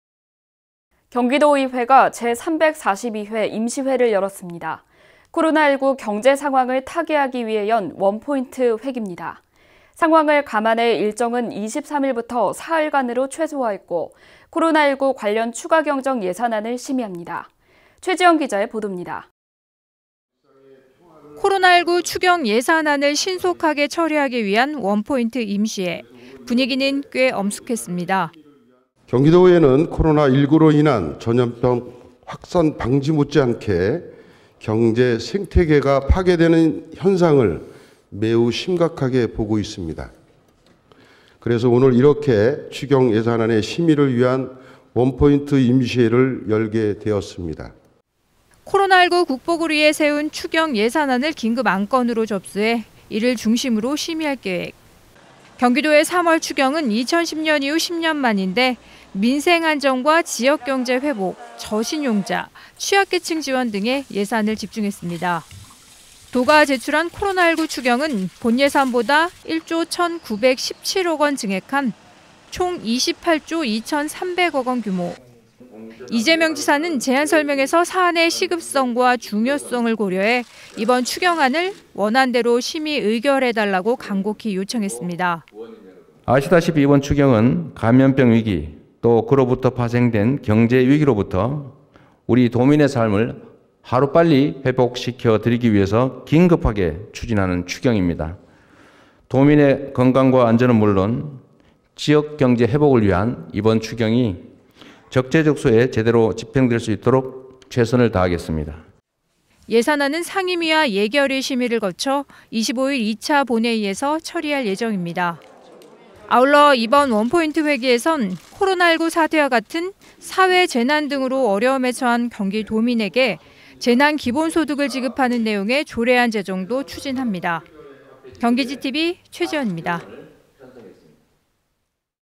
23일부터 코로나19 추경 원포인트 임시회
[앵커멘트]
경기도의회가 제342회 임시회를 열었습니다. 코로나19 경제 상황을 타개하기 위해 연 ‘원포인트’ 회기입니다. 상황을 감안해 일정은 23일부터 사흘간으로 최소화했고, 코로나19 관련 추가경정예산안을 심의합니다. 최지현 기자의 보도입니다.

[리포트]
1.[자막]제342회 임시회(경기도의회, 3월23일)
코로나19 추경예산안을 신속하게 처리하기 위한 원포인트 임시회.
분위기는 꽤 엄숙했습니다.

2.[자막][싱크]송한준 경기도의회 의장
“경기도의회는 코로나19로 인한 감염병 확산 방지 못지않게 경제 생태계가 파괴되는 현상을 매우 심각하게 보고 있습니다. 그래서 오늘 이렇게 추경예산안의 심의를 위한 원포인트 임시회를 열게 됐습니다.”

3. [자막]3.코로나19 추경 중심 심의
코로나19 극복을 위해 세운 추경예산안을 긴급 안건으로 접수해, 이를 중심으로 심의할 계획.

경기도의 3월 추경은 2010년 이후 10년 만인데 민생안정과 지역 경제 회복, 저신용자, 취약계층 지원 등에 예산을 집중했습니다.

4.[자막]경기도, 1조 1천917억 원 규모 코로나19 추경 편성…
도가 제출한 코로나19 추경은 본예산보다 1조 1천917억 원 증액 한 총 28조 2천3백억 원 규모.
이재명 지사는 제안 설명에서 사안의 시급성과 중요성을 고려해 이번 추경안을 원안대로 심의 의결해달라고 간곡히 요청했습니다.

5.[자막][싱크]이재명 경기도지사
“아시다시피 이번 추경은 감염병 위기, 또 그로부터 파생된 경제 위기로부터 우리 도민의 삶을 하루빨리 회복시켜 드리기 위해서 긴급하게 추진하는 추경입니다. 도민의 건강과 안전은 물론 지역 경제 회복을 위한 이번 추경이 적재적소에 제대로 집행될 수 있도록 최선을 다하겠습니다.”

6.[자막]25일 처리 예정
예산안은 상임위와 예결위 심의를 거쳐 25일 2차 본회의에서 처리할 예정입니다.

아울러 이번 원포인트 회기에선 코로나19 사태와 같은 사회재난 등으로 어려움에 처한 경기도민에게 재난기본소득을 지급하는 내용의 조례안 제정도 추진합니다. 경기GTV 최지현입니다.

영상취재: 이효진, 나인선, 김현우, 영상편집: 김상환